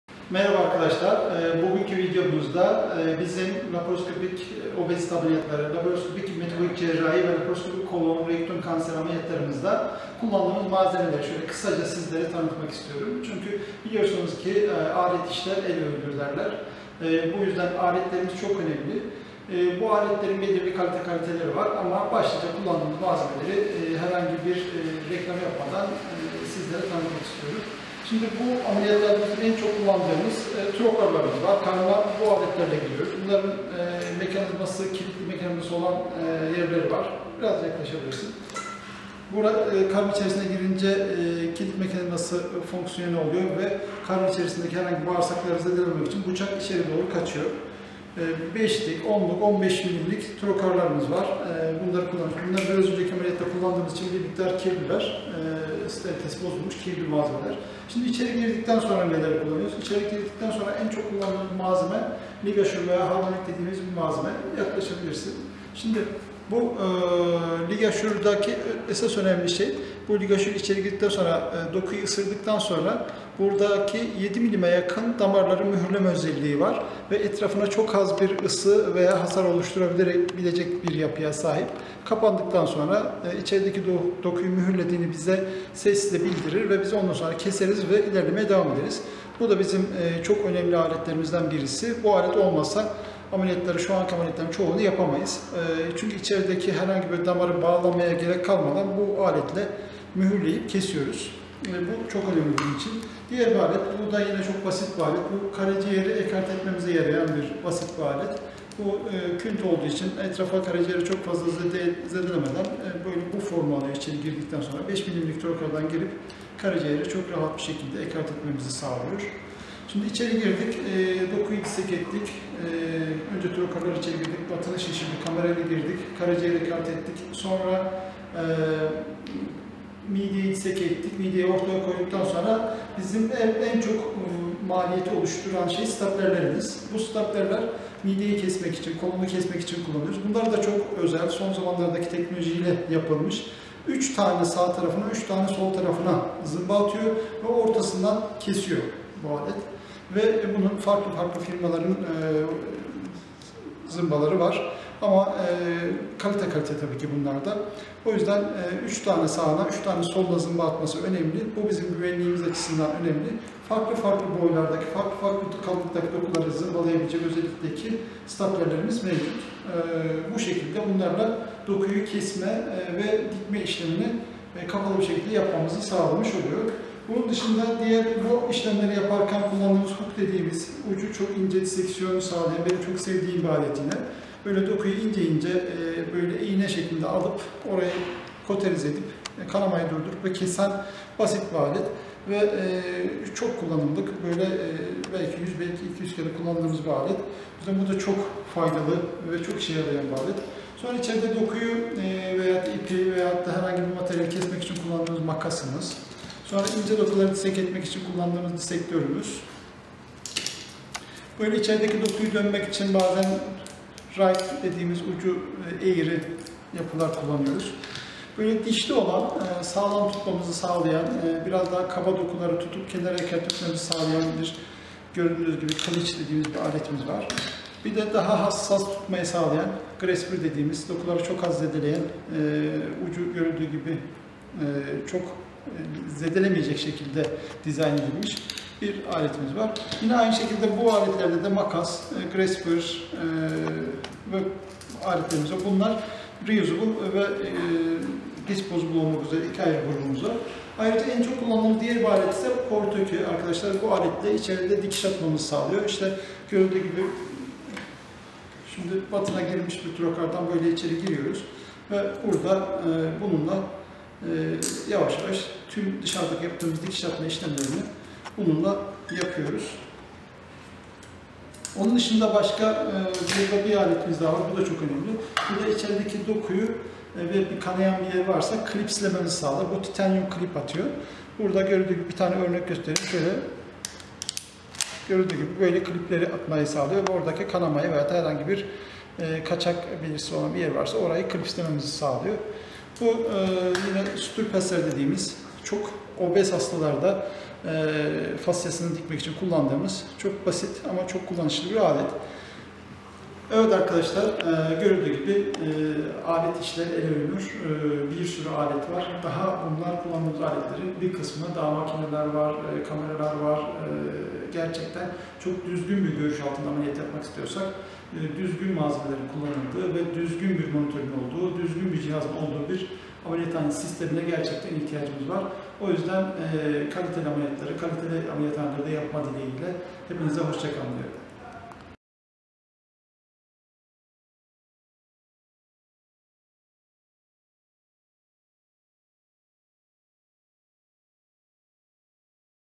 obezite ameliyat aletleri
Atalarımız; alet işler el övünür derler, çok doğru söylerler  bizim obezite ameliyatlarında da aletlerimiz ve aletlerin kalitesi ameliyat başarısında çok önemlidir.